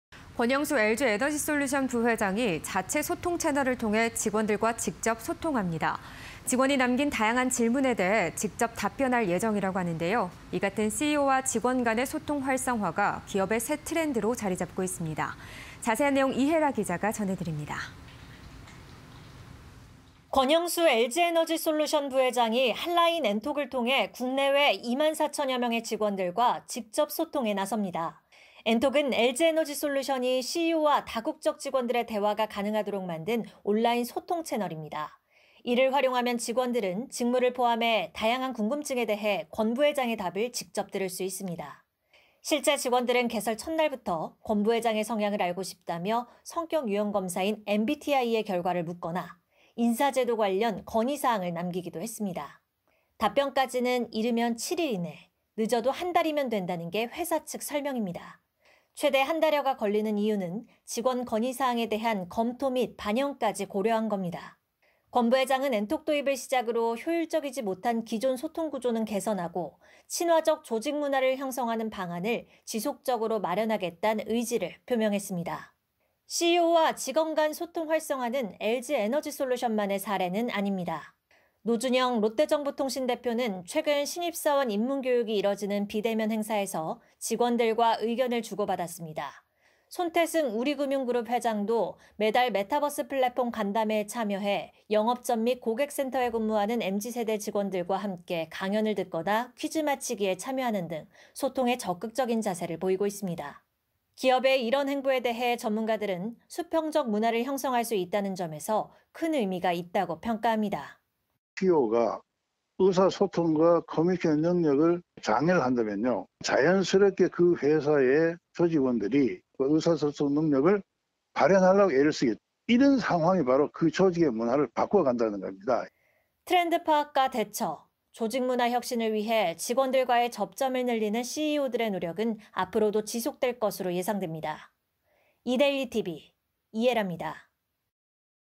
권영수 LG엔솔 부회장, 직원과 핫라인 ‘엔톡’ 개설 (20211116)
권영수 LG엔솔 부회장, ‘엔톡’서 직원과의 직접 소통 방식 ‘눈길’
- 7일 이내 답변… 제도 개선·반영시 한 달여 내 응답
- CEO-임직원 ‘직접 소통’ 사례 늘어… “MZ 접점 늘린다”